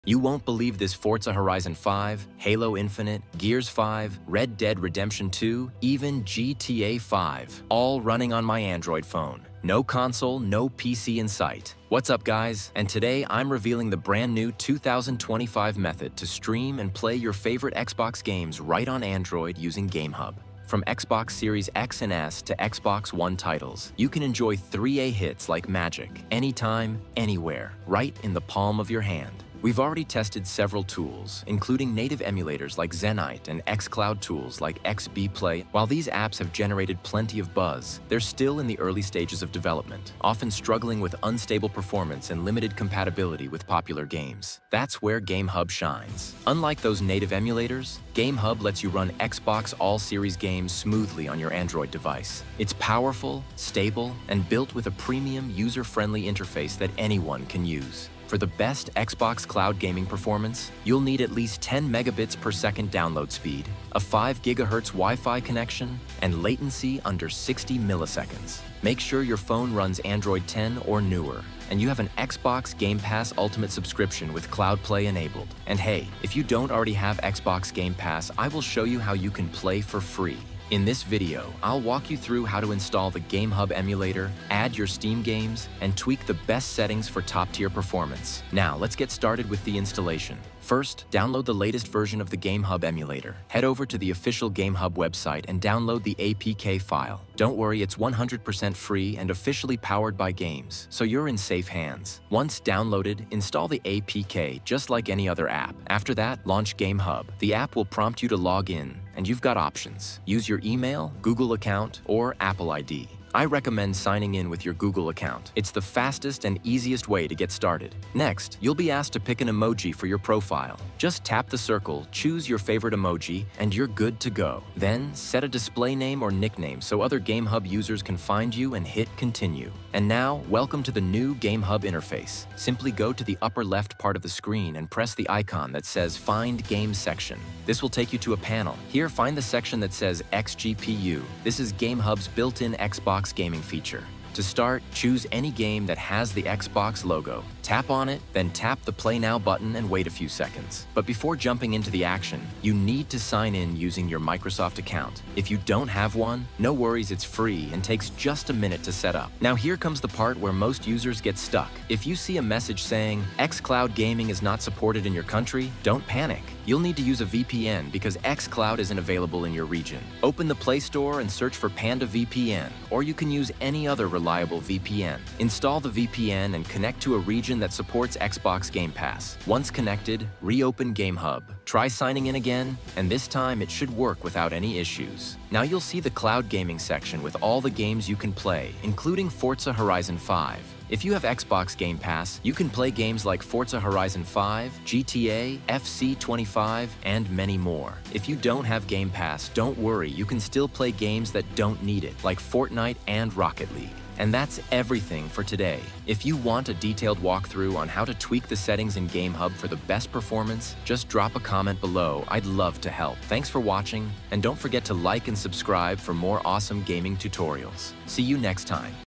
How to Play XBOX Games on Any Android Device with GameHub
Chapters:
00:00 Xbox Game on Android
00:31 Xbox Games on GameHub
01:33 Download & Install GameHub
01:53 Setup GameHub
02:27 Connect with Xbox
02:40 Bypass Region
03:10 GameHub Xbox Gameplay